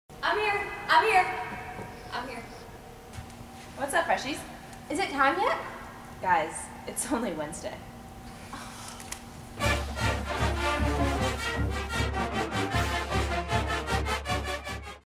Alabama Gymnastics: Is it Time Yet? (Wednesday)
Alabama Gymnastics' freshmen, including newcomers Aja Sims and Keely McNeer, are more than ready to get things started, so their only question for junior All-American Kayla Williams is "Is it time yet?"